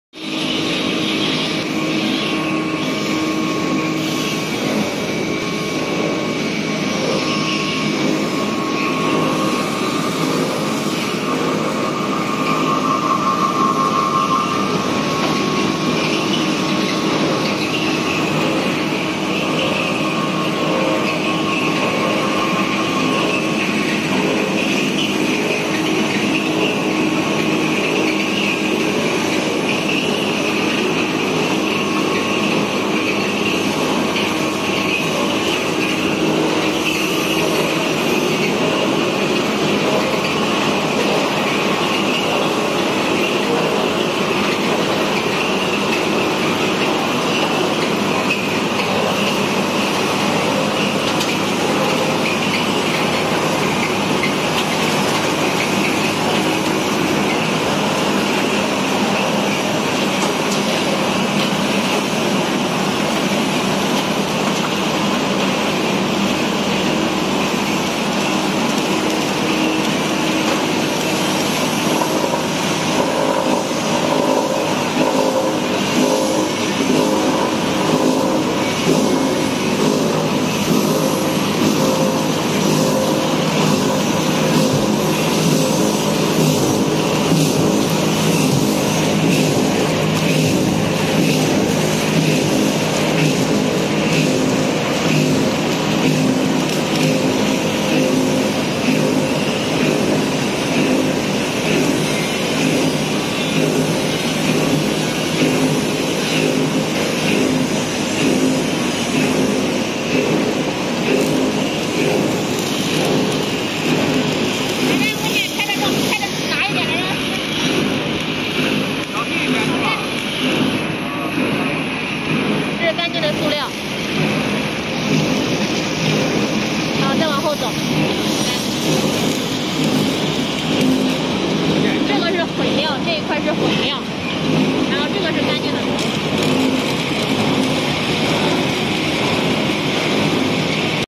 Jelly Filled Telephone Cables Recycling Machine | Wet Type Cable Granulator
How to recycle jelly filled telephone cables? Wet type cable granulator made by Henan Recycle. Cleaning by special detergent, and water separation device. We can easily recycle jelly-filled cables. Capacity is 500-1000kg/h.